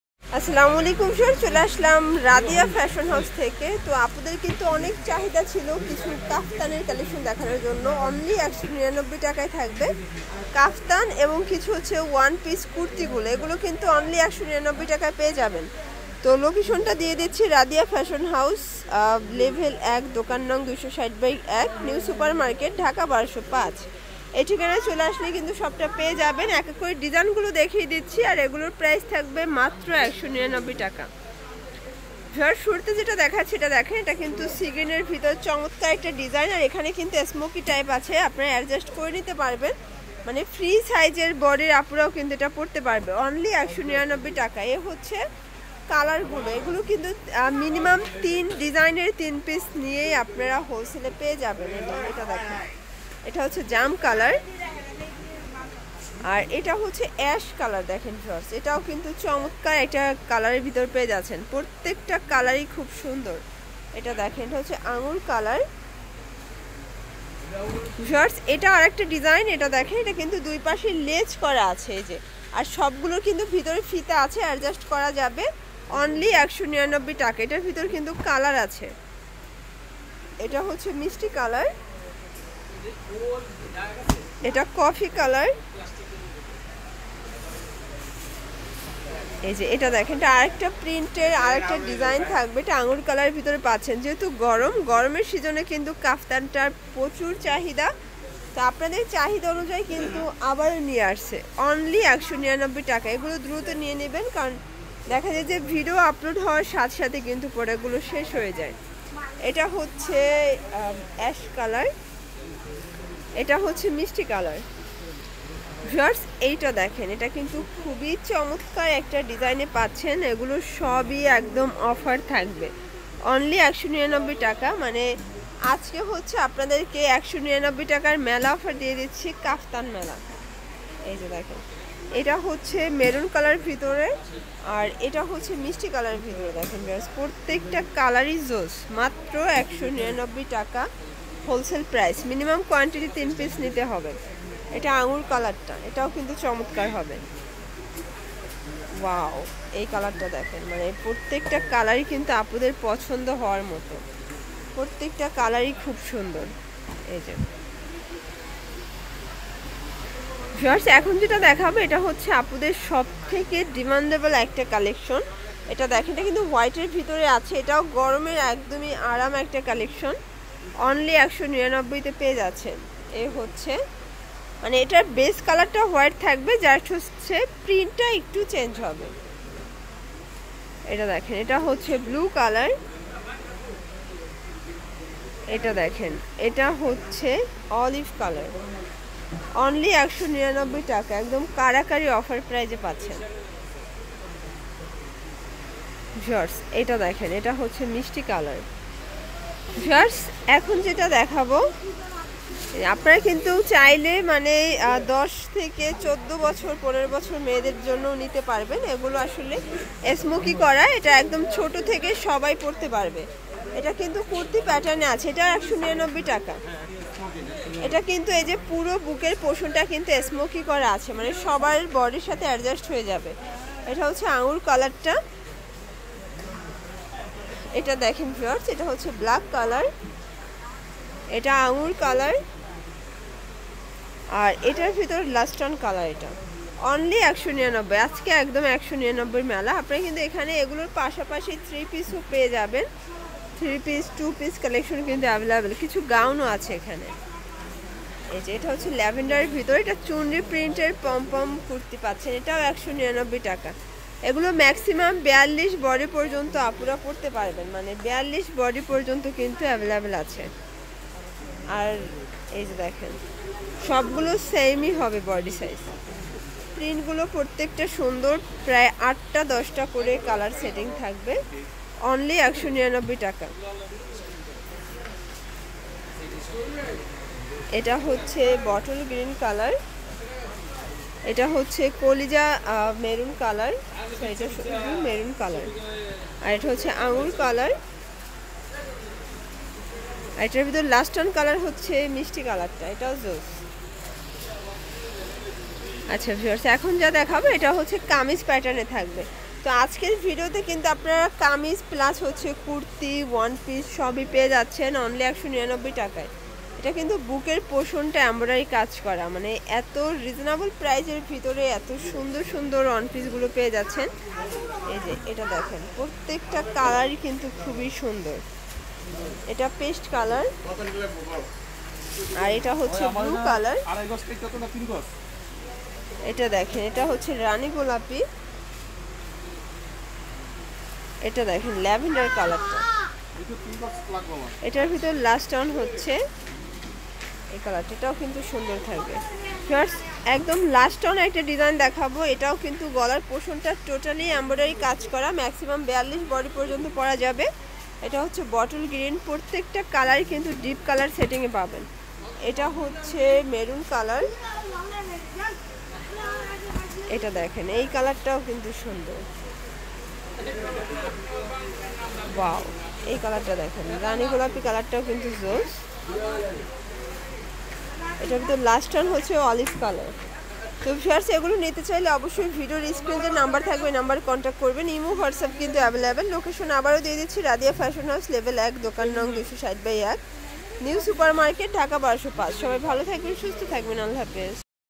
199 টাকায় কাফতান মেলা / পোড়া দোকান থেকে আবারো নতুন ভাবে শুরু করলেন
199 টাকায় কাফতান মেলা / পোড়া দোকান থেকে আবারো নতুন ভাবে শুরু করলেন